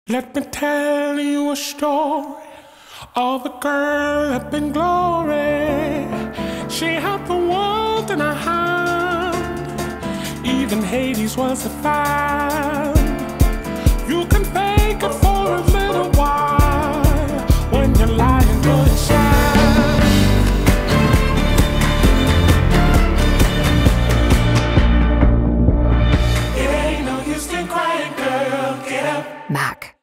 Meet NEW Studio Radiance Serum-Powered Foundation | MAC Cosmetics
Get into MACStudioRadiance SerumPowered Foundation ✨⚡. Meet the serum-powered foundation that fuses our expertise of skin tones with the science of skincare. It delivers +209% instant hydration for plumper, smoother and more radiant skin. Shop 56 luminous shades for all.

Follow MACCosmetics on social